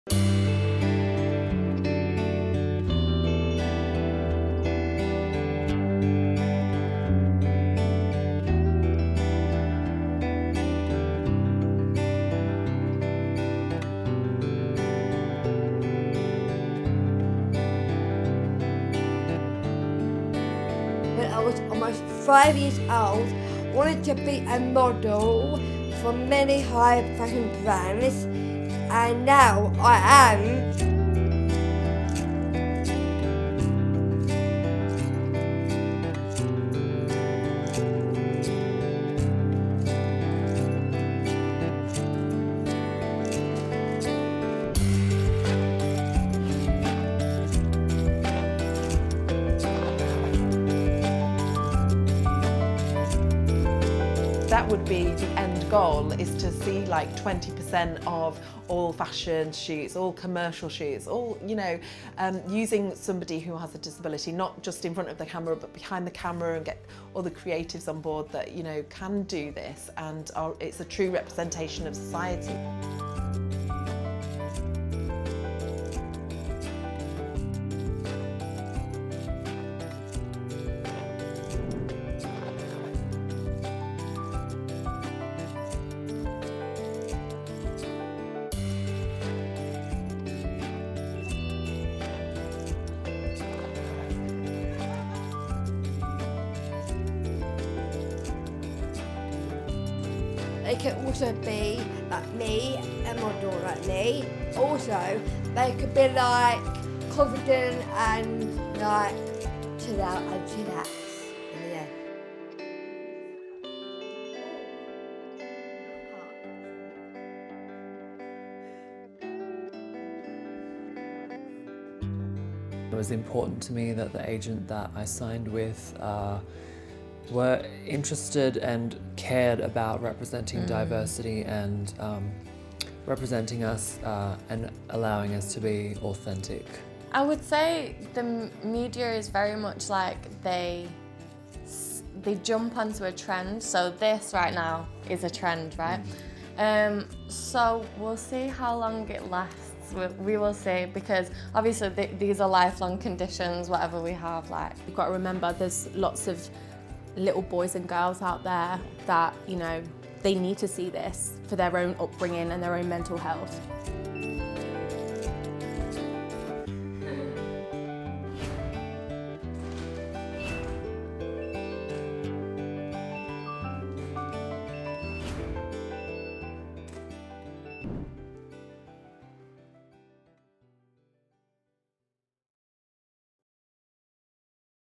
Modelo con síndrome de Down cumplió su sueño
En el Día Internacional de la Diversidad Biológica, DW nos presenta a Ellie Golstein, una exitosa modelo global y la protagonista del post más popular de Gucci en Instagram. Ser Síndrome de Down no le impidió cumplir su sueño.